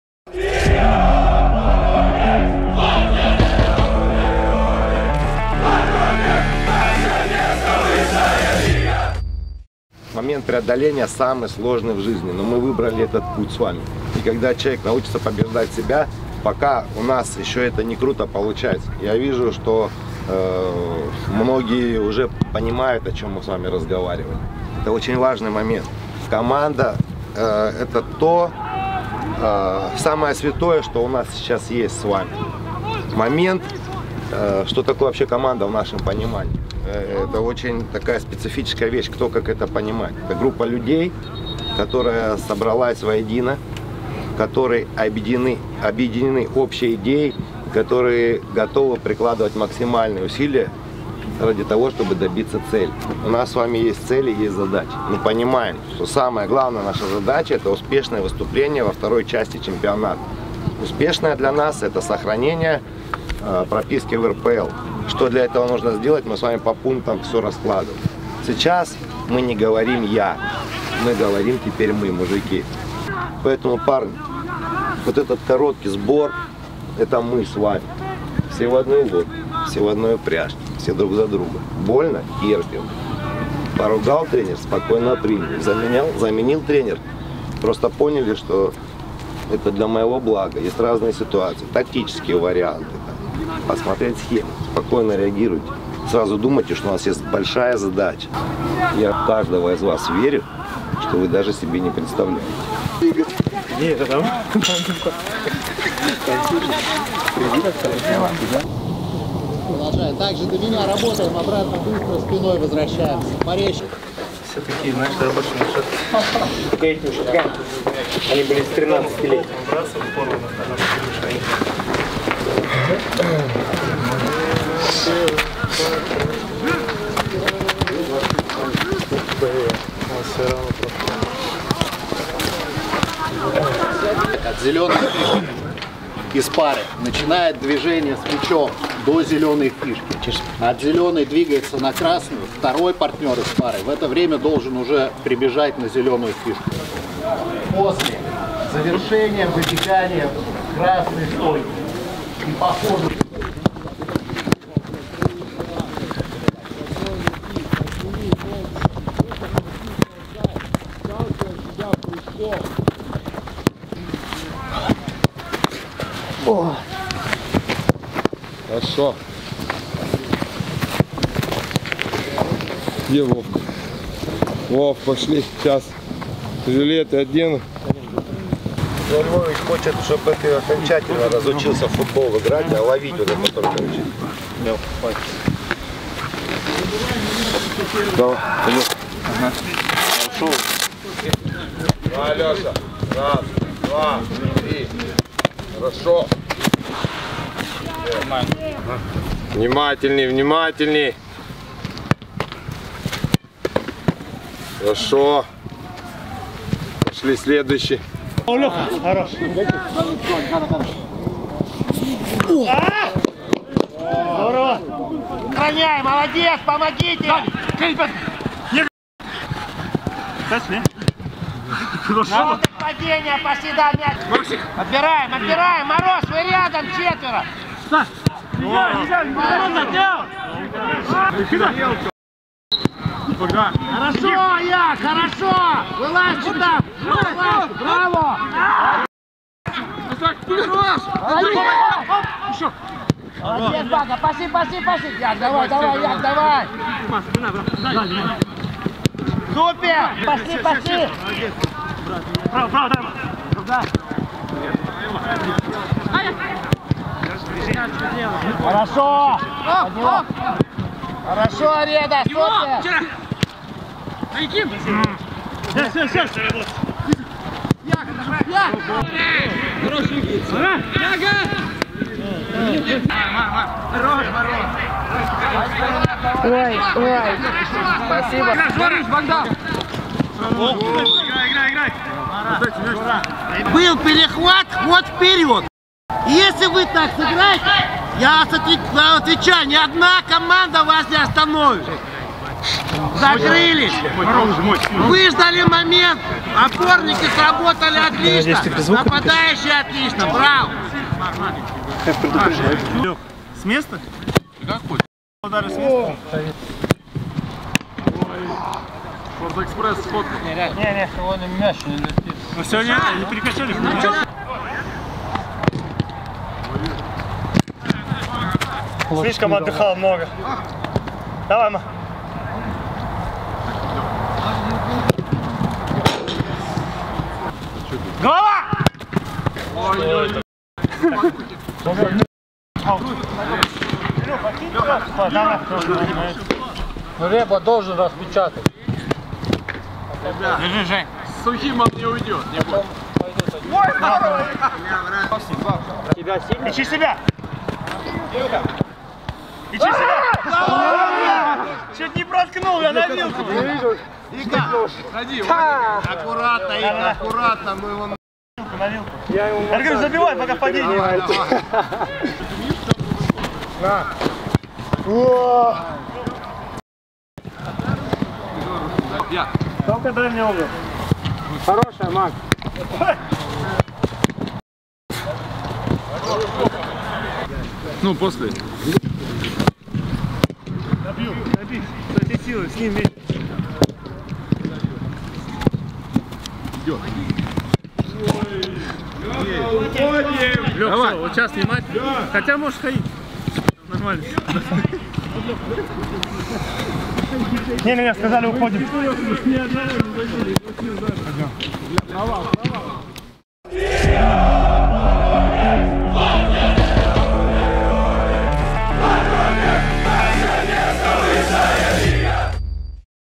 «Факел»-22/23 | Старт третьего зимнего сбора
Стартовал третий сбор. Остаётся всё меньше времени до возобновления сезона. Как команда готовится к старту весеннего отрезка МИР РПЛ - в нашем видео.